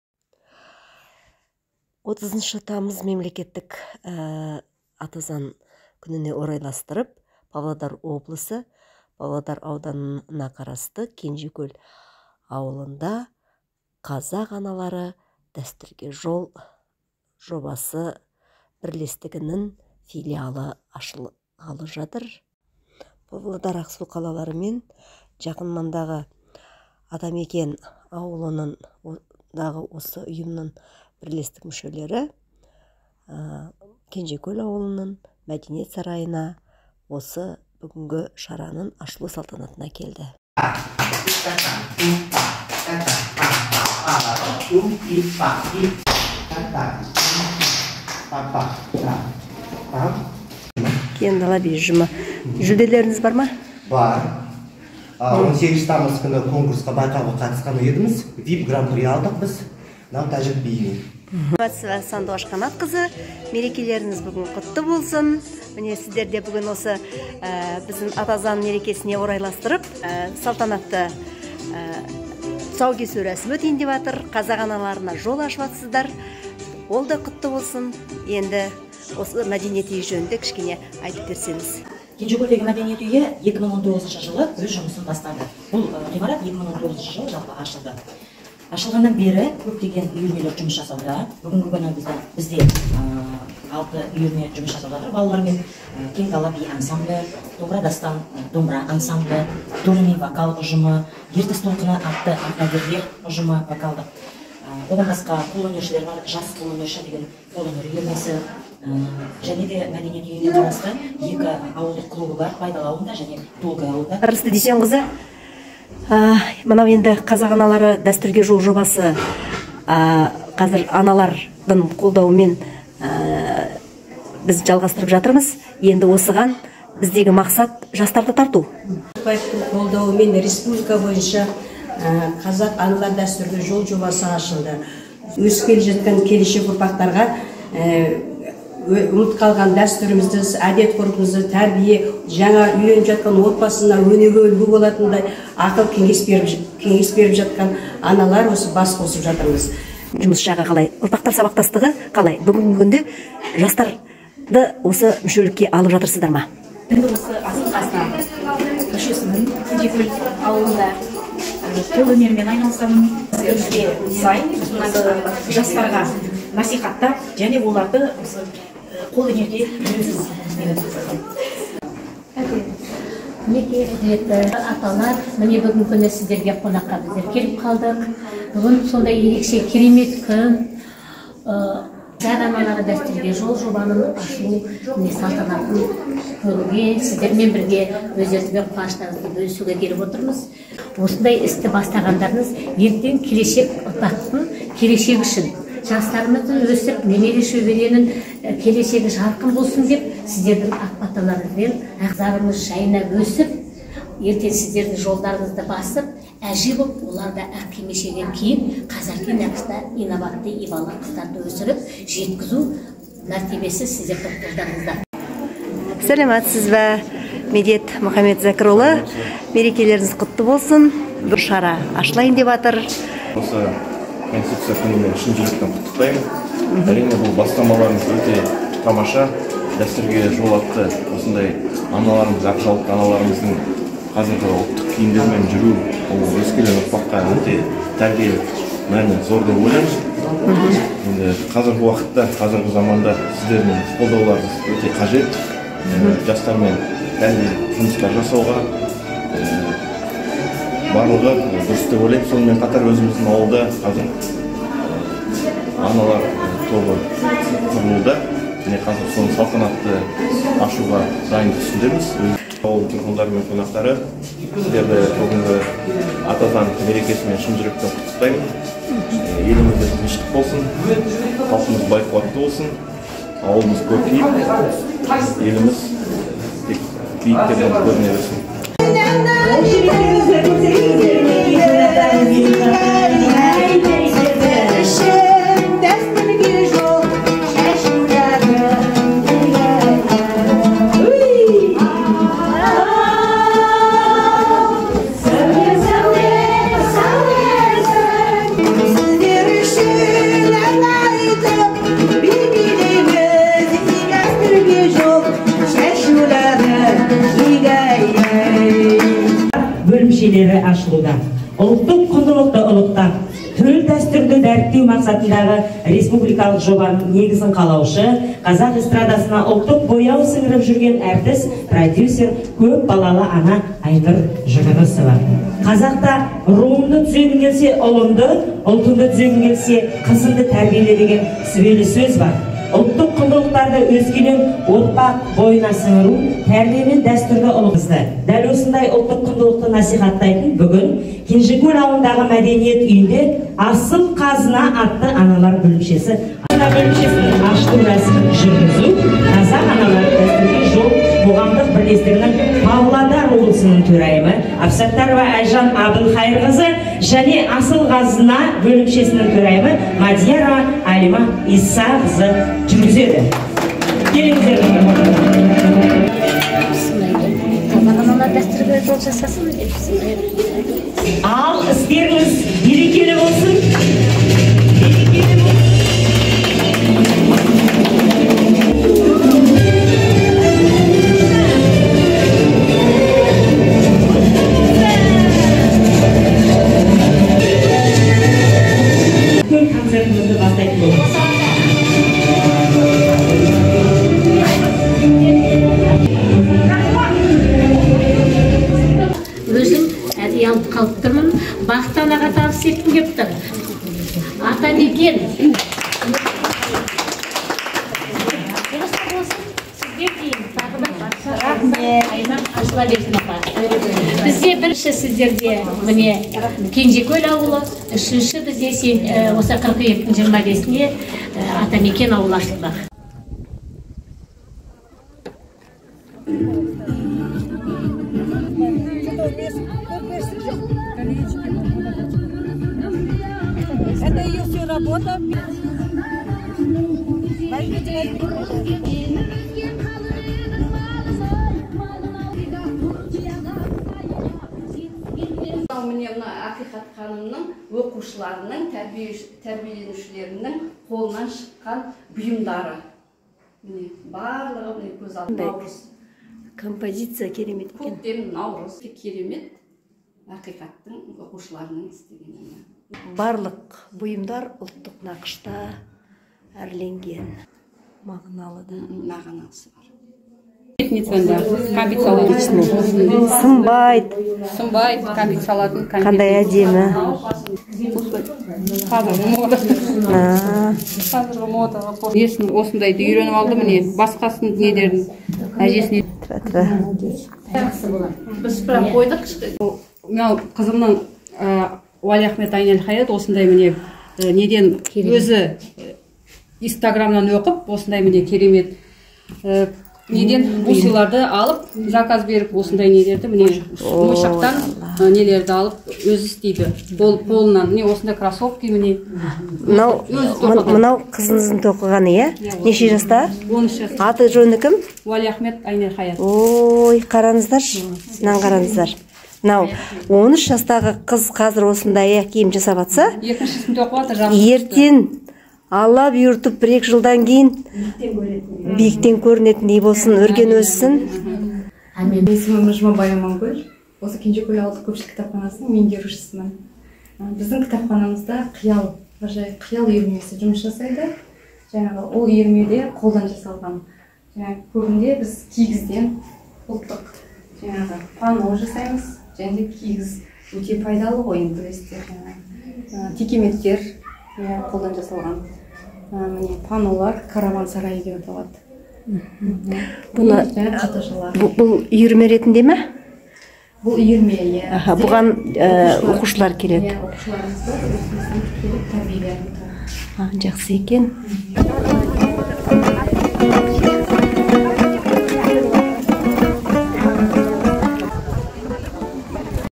"Асыл казына" - "Казак аналары-дастурге жол" уйымы.Павлодар облысы
Павлодар облысы, Павлодар ауданы,Кенжекол ауылында "Казак аналары-дастурге жол" уйымы "Асыл казына" такырыбымен ашылды.Торайымы - Адиба Исаева.